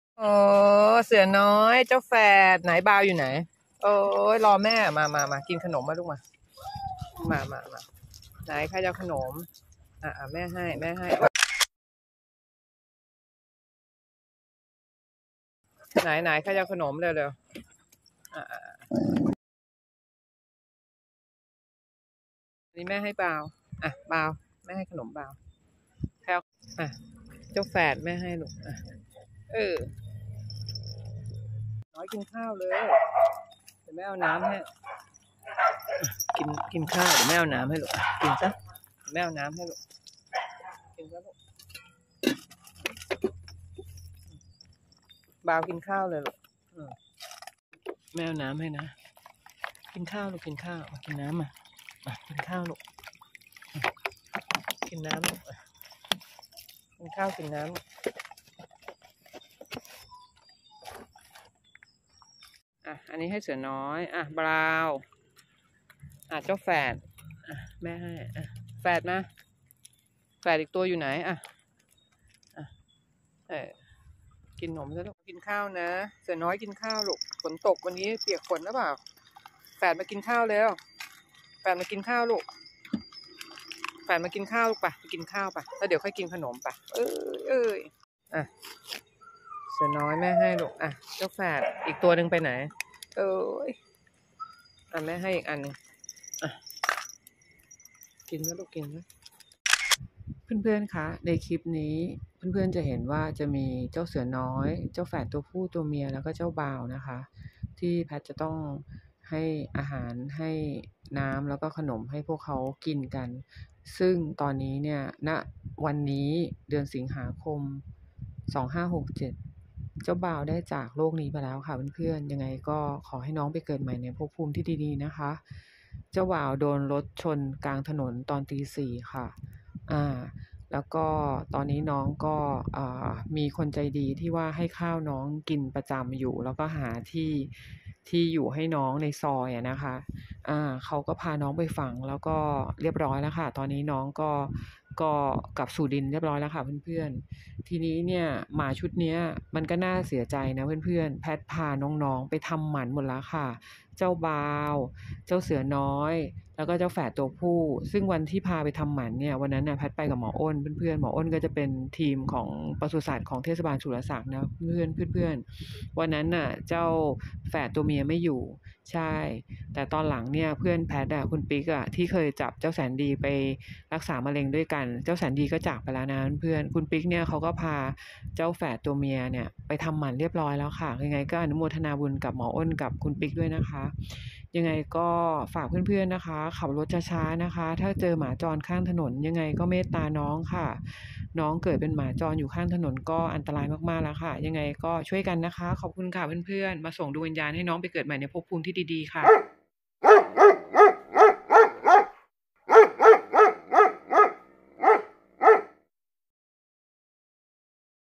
ได้บุญ♥️โปรดขับรถช้า ๆ หากพบเจอหมาจรเดินข้างถนน#หมา
เจ้าบราวน์_หมาจรที่อยู่ในกลุ่มเจ้าเสือน้อยดูในคลิปนะคะ_ตอนนี้ได้จากโลกนี้ไปแล้วค่ะ_น้องถูกรถชนตุยค่ะ_ขอให้เจ้าบราวน์ได้ไปเกิดใหม่ในภพภูมิที่ดี ๆ สาธุค่ะ🙏♥️🐶หมดเวรหมดกรรมนะลูกเอ๊ย

คลิปนี้ถ่ายเมื่อวันที่ 18 มีนาคม 2567
คลิปนี้ลงเผยแพร่ 19 สิงหาคม 2567

ได้โปรดขับรถช้า ๆ นะคะหากเจอน้องหมาอยู่ข้างถนน ทุกชีวิตมีค่าค่ะ
น้องหมาไม่รู้จักหลบรถ_โปรดเมตตากับสัตว์ผู้น่าสงสารข้างถนนด้วยนะคะ_กรุณาขับช้า ๆ ค่ะ 🙏สาธุบุญค่ะ